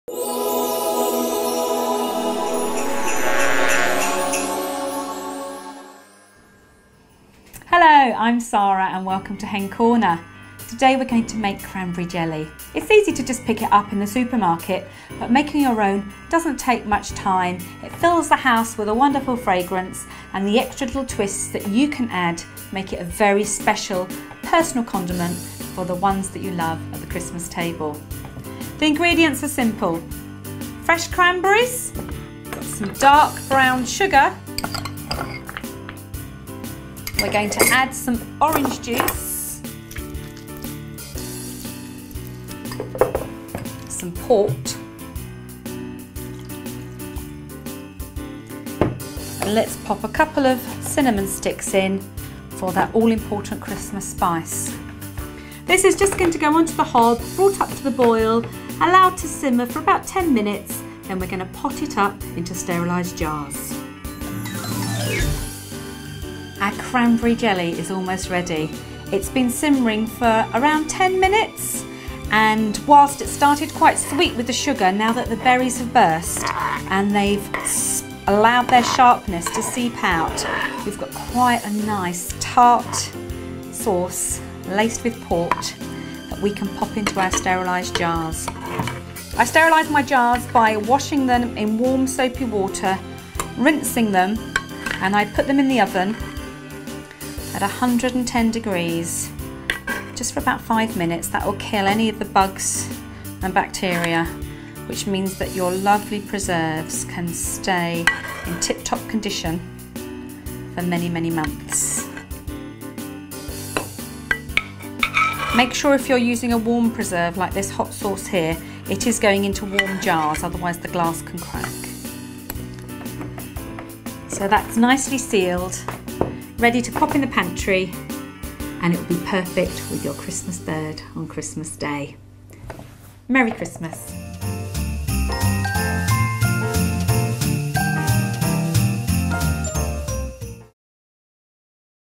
Hen Corner Cranberry Sauce
Sara Ward of Hen Corner shows you how easy it is to make fresh cranberry sauce. This is perfect for Christmas or any special roast dinner throughout the year, it also pairs very well with Brie in her Filo Parcels